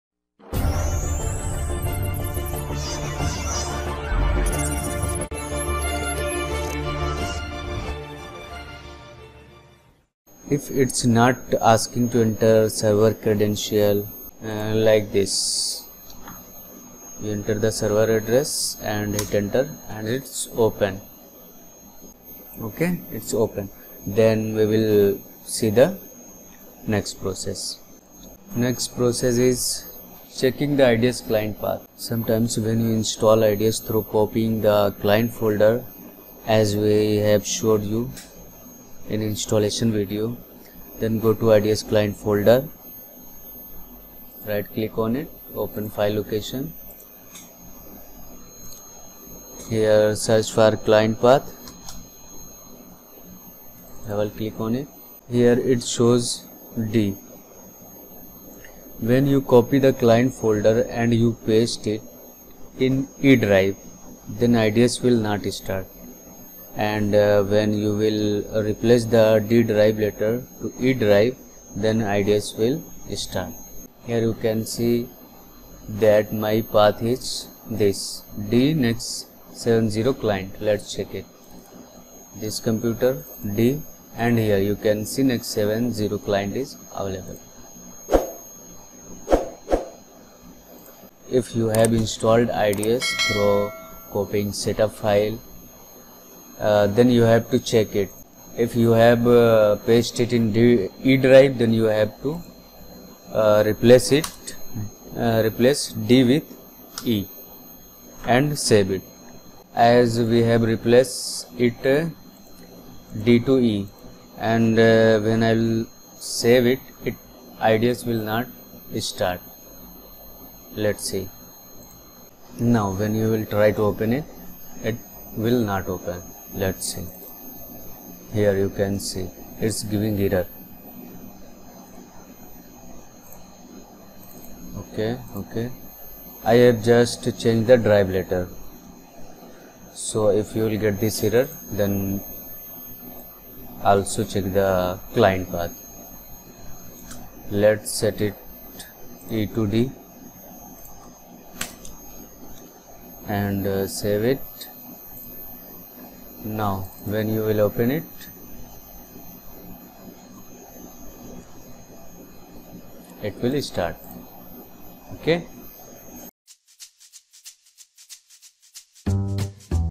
IDS Training | Full Training of IT Department | Troubleshooting | IDS is not Starting in English.
In this video, I am going to show you How to Troubleshoot if IDS is not starting in English.